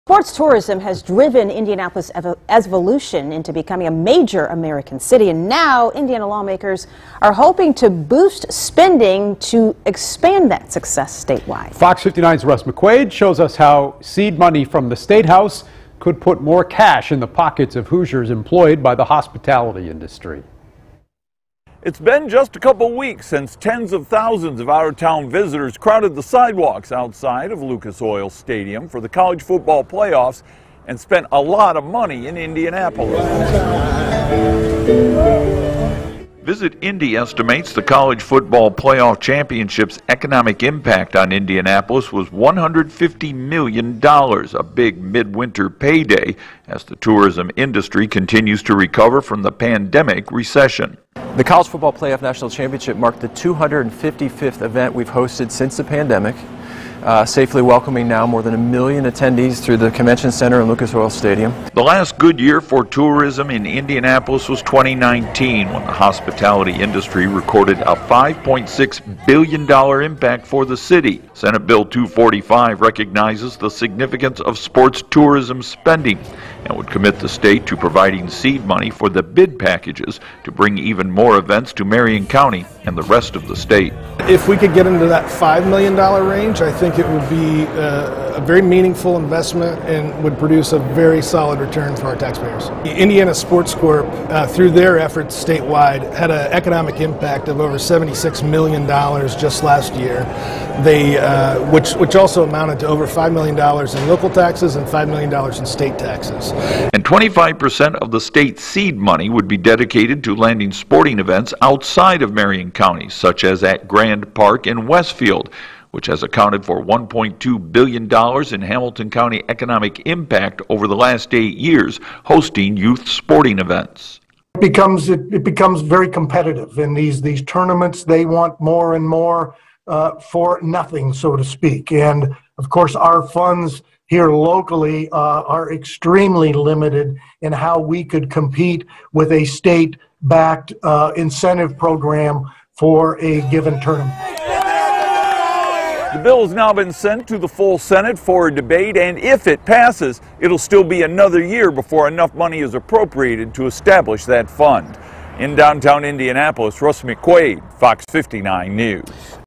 Seed money for landing sporting events advances to Indiana Senate
When the final books are balanced in the next sixty days, it's estimated that Georgia and Alabama fans and college football followers will have left behind an estimated $150 million economic impact on Central Indiana as a result of this month’s College Football Playoffs Championship game.